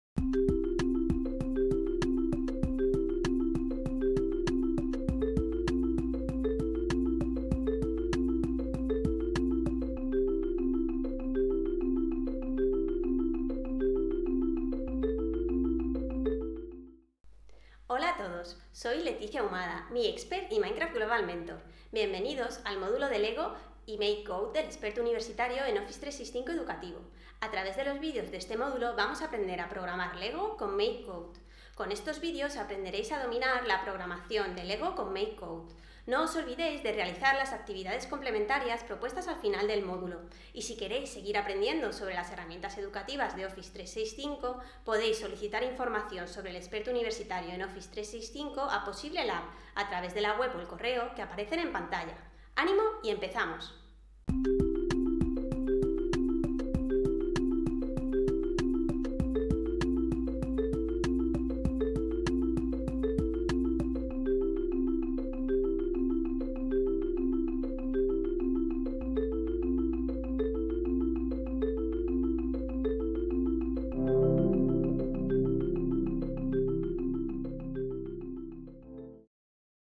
MakeCode y Lego - Experto O365
Bienvenidos al módulo de Lego y MakeCode del experto universitario en Office365. Completa el itinerario de aprendizaje y aprende a programar EV3 con MakeCode.

INNOBITES